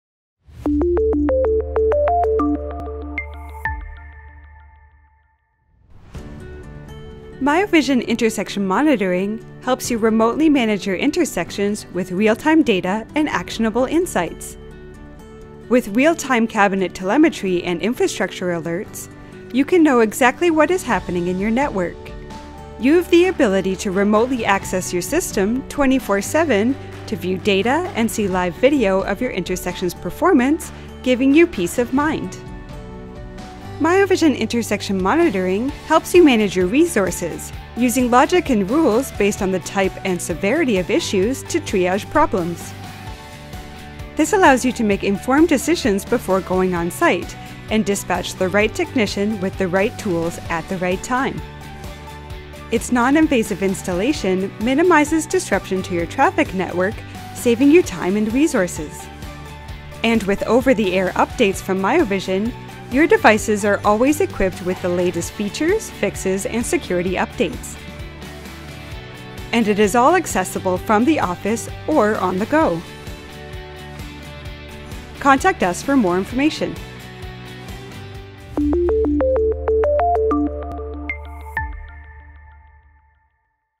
Remote Intersection Monitoring & Management | Miovision TrafficLink
Learn how Miovision Intersection Monitoring empowers traffic teams to manage their network remotely, reducing unnecessary field visits and improving response times.

In this video, we explore how real-time cabinet telemetry and infrastructure alerts give you 24/7 visibility into your intersections. Discover how to triage problems from your office—viewing live video and performance data—so you can dispatch the right technician with the right tools only when necessary. Plus, see how our non-invasive hardware installs easily and stays secure with automatic over-the-air updates.

Key Features & Benefits:

Remote Visibility: Access live video and real-time data from your intersections 24/7.

Smart Alerts: Receive instant notifications for cabinet faults and infrastructure issues.

Efficient Dispatch: Triage issues remotely to ensure technicians arrive prepared, saving time and resources.

Easy Maintenance: Benefit from non-invasive installation and automatic security updates.

⏱️ Timestamps
0:00 – Introduction to Remote Intersection Management
0:15 – Real-Time Data & Infrastructure Alerts
0:23 – 24/7 Remote Video Access
0:35 – Triaging Issues & Optimizing Technician Dispatch
0:52 – Non-Invasive Installation & Over-the-Air Updates